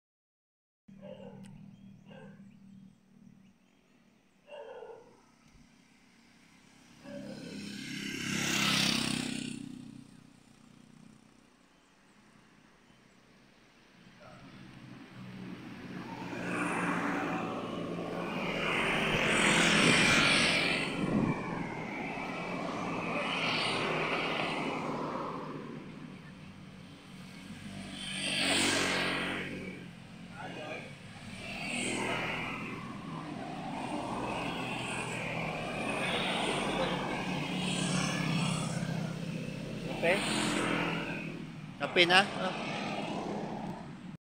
Santa Maria near Infanta arc boundary
manila temperature was around 34 degree C while in this area is 22 degree C @ 6 am.